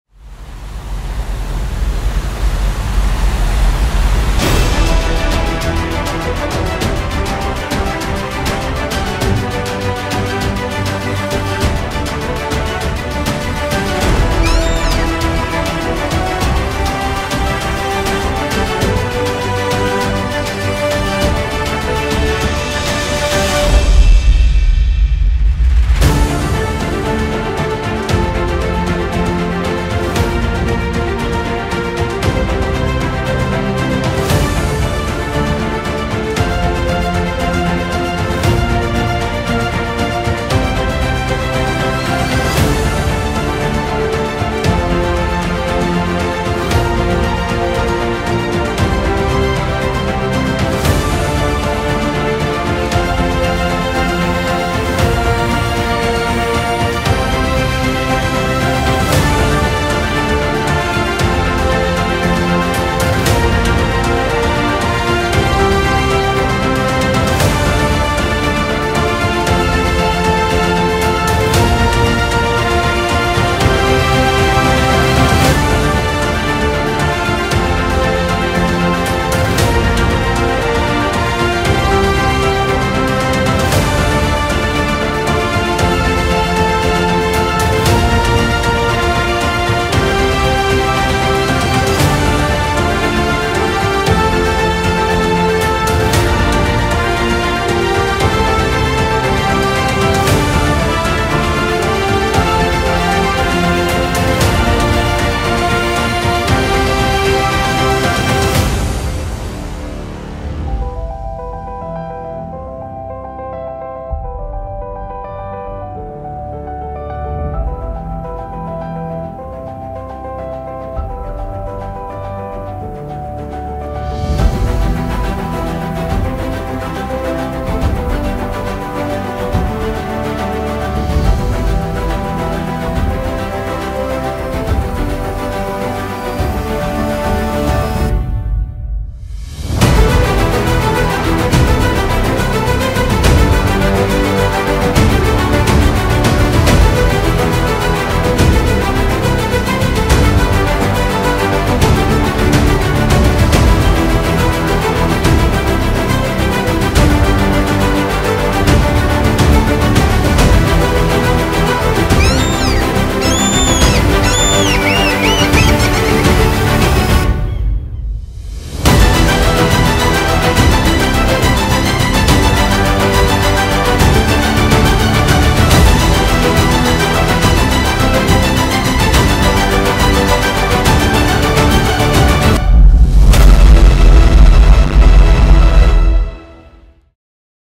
Tersan Shipyard has launched three vessels at one time: Havila Polaris& Havila Pollux and Polar Bay.
Congrats to all Tersan team who made this story possible! Here is the launching story of three vessels

Tersan Tersanesi aynı anda üç gemiyi denize indirdi: Havila Polaris & Havila Pollux ve Polar Bay.

Bu hikayeyi mümkün kılan tüm Tersan ekibini tebrik ediyoruz. İşte üç geminin aynı anda denize iniş hikayesi!